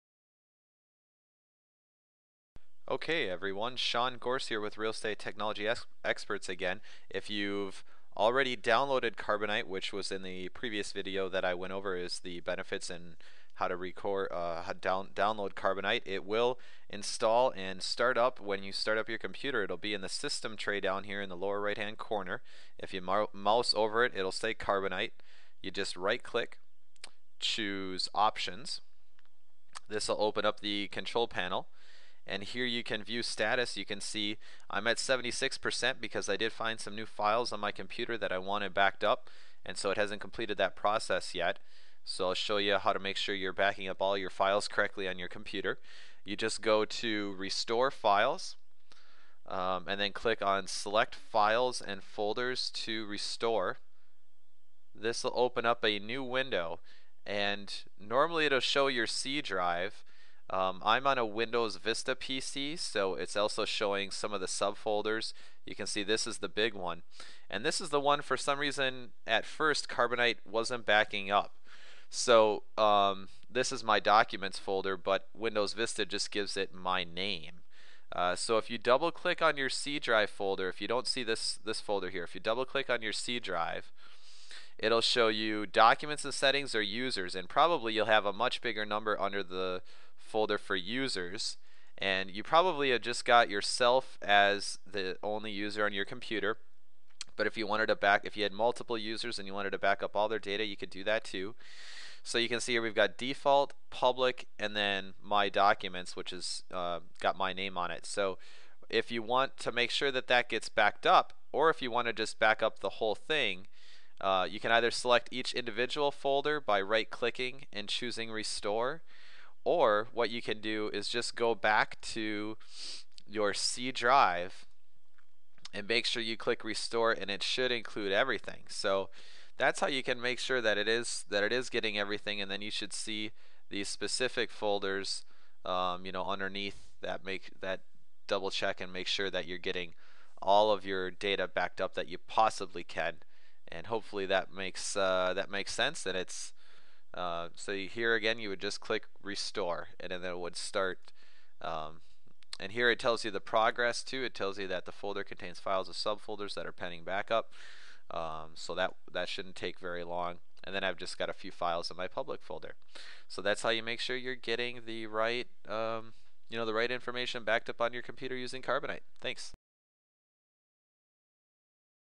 Carbonite Backup - Part 2 of 3 - Make sure it backs up EVERYTHING
The default Carbonite settings are less than desireable - here's what you'll need to tweak to make SURE that you get EVERYTHING you want backed up on your computer. Also, I'm linking to this fifteen day trial so you don't have fork over the money up front, and you can make sure it's something that's a good fit. I haven't heard of anyone disliking this software once they get it going. Post a comment if you have a question!